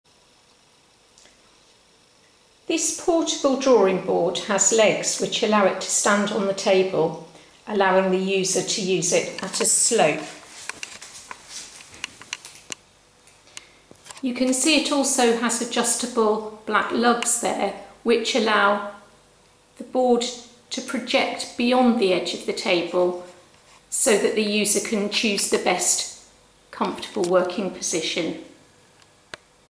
How to set up and use an adjustable drawing board
Short clip on adjustable drawing boards and setting one up.